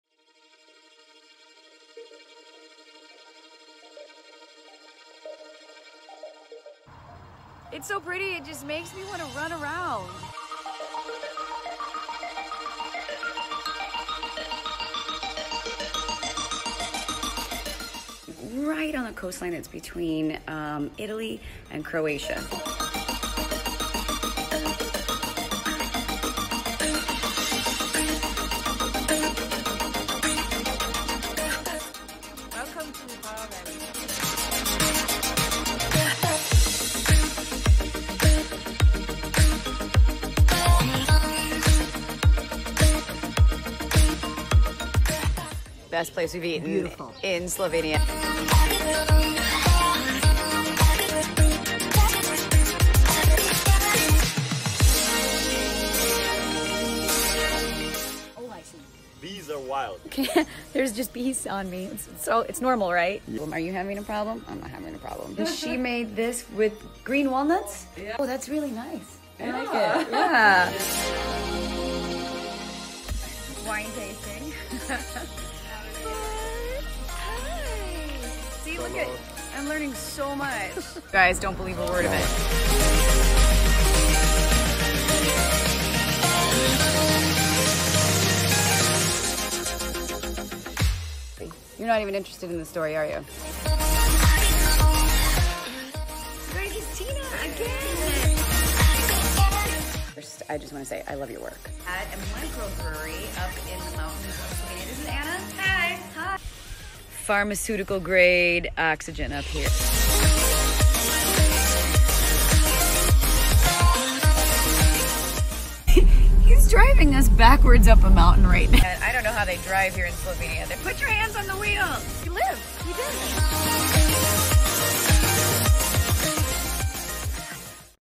Travel to Slovenia with Juliana Dever
Take a Culturally Immersive tour of Slovenia with Juliana Dever of CleverDever Wherever